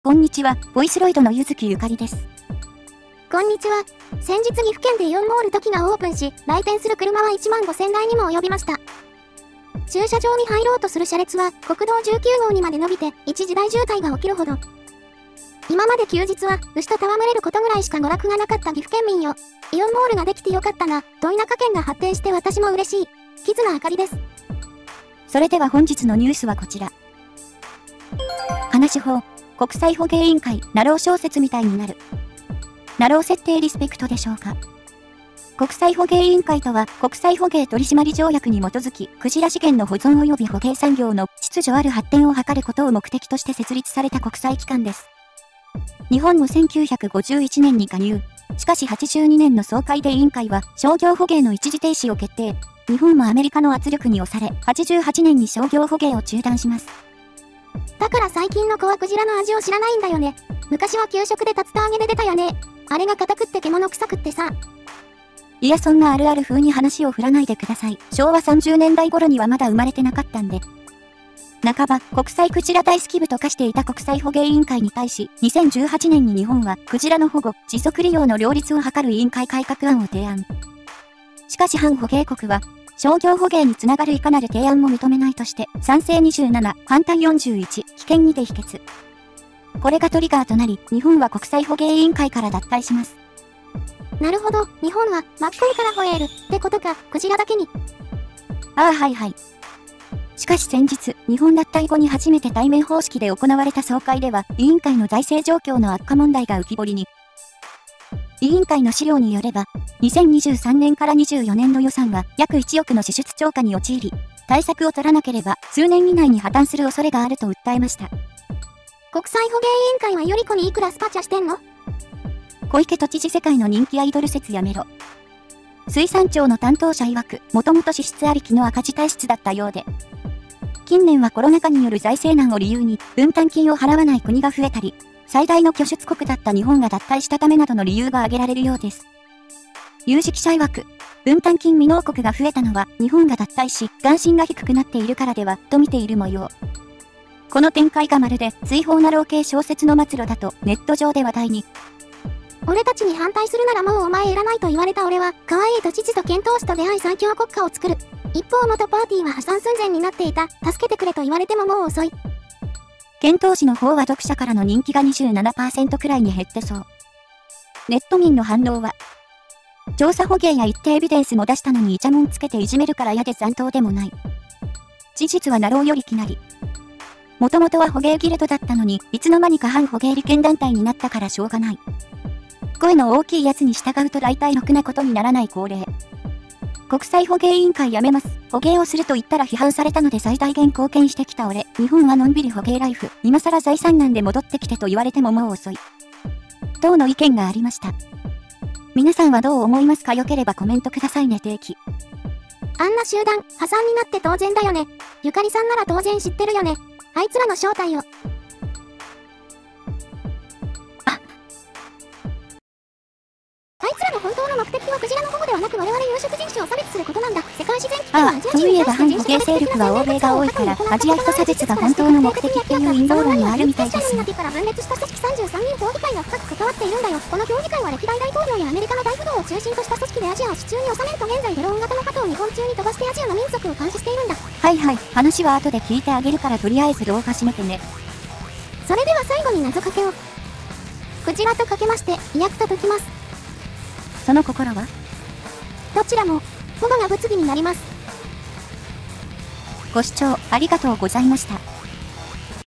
日本が脱退した後の国際捕鯨委員会、追放なろう系みたいな結果になる
鯨の大和缶は正直あまりおいしくない…　ただタンパク質は豊富だから防災用に少しだけストックしてます
豚カツかニンニクを支援してくれる人
素材はこちらのサイトからお借りしております
みんなもVOICEROID買って動画作ろう！